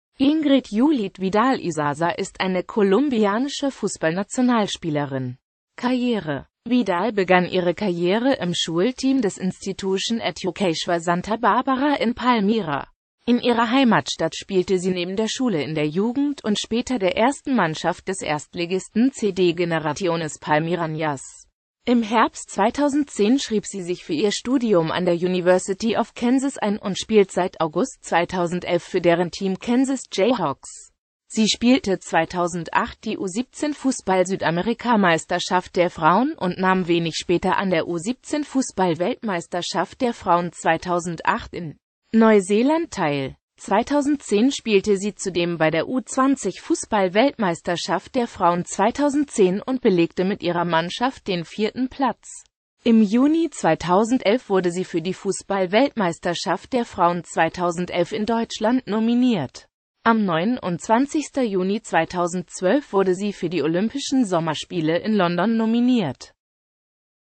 Ingrid Vidal
Vielen Dank für Ihre Unterstützung:

Ingrid Yulieth Vidal Isaza (* 22.April 1991 in Palmira) ist eine kolumbianische Fußballnationalspielerin.